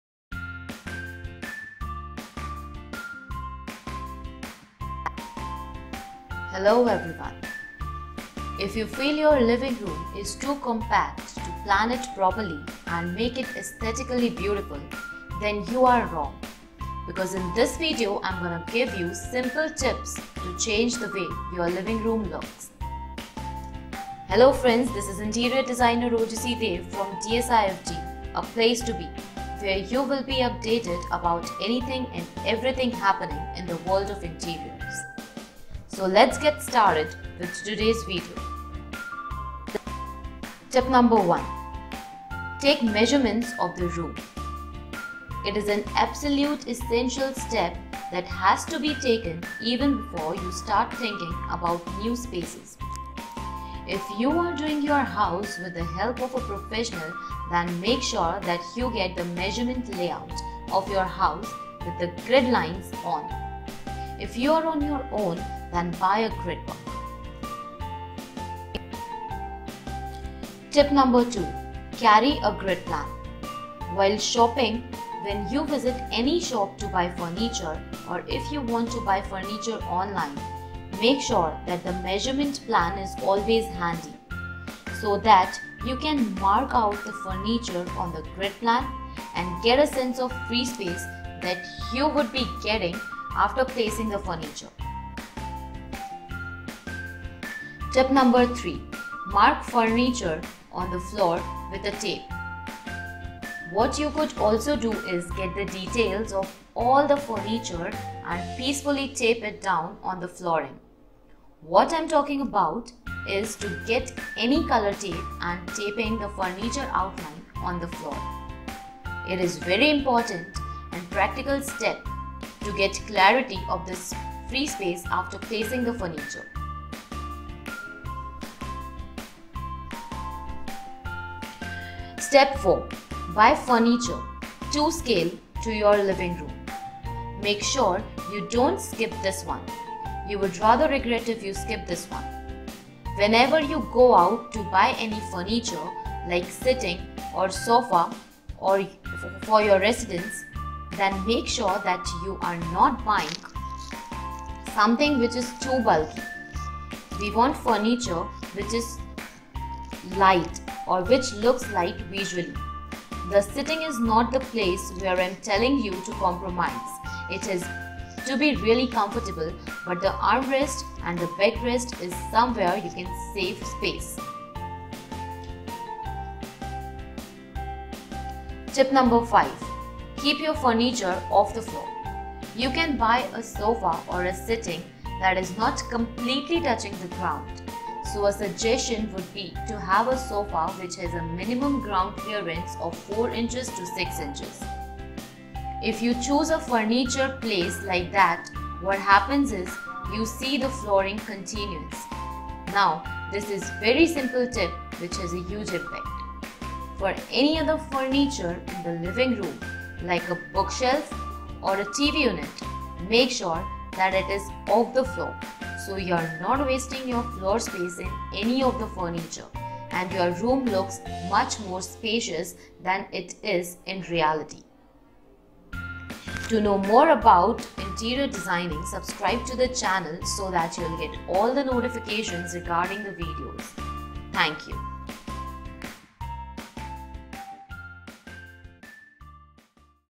Online Interior Designing Course for Beginners | Top 10 Living Room Design Tips | DSIFD Indore
DSIFD Indore Starts Online Interior Designing Course for Beginners. Lets Find Out the First Part of Top 10 Living Room Design Tips.
For More Details About the Courses and Admission
Be in Touch with Us.